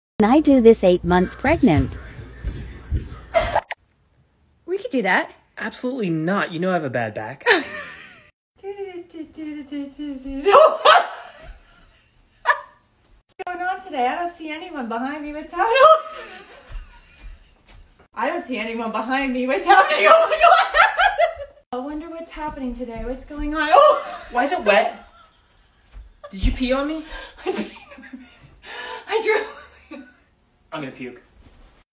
tiktok challenge 2021 - crotch lift - couple goals challenge
The “Crotch Lift” challenge takes control of TikTok with men trying to lift their partners with a single arm placed (you guessed it) between their legs.

🎯 Call of shorts 🎯

Viral, trends, trending, challenge 2021, tiktok dance 2021, vines, tiktok compilation, tiktok meme, tiktok mashup